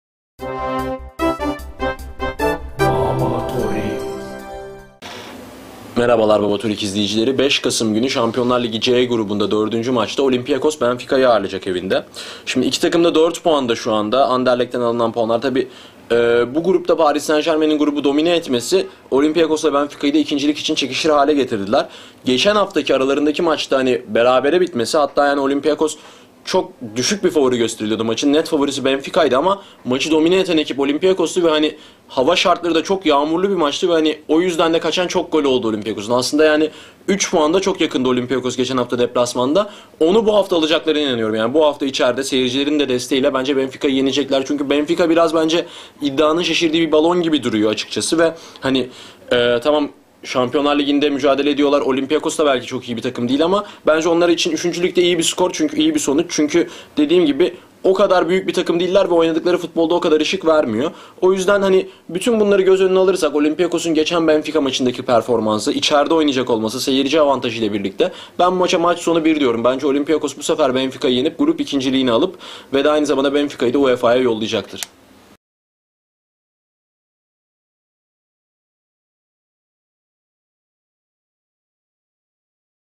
Babatorik Olympiakos Benfica 5 Kasim 2013
guney anlatiyor 5 Kasim 2013 Olympiakos Benfica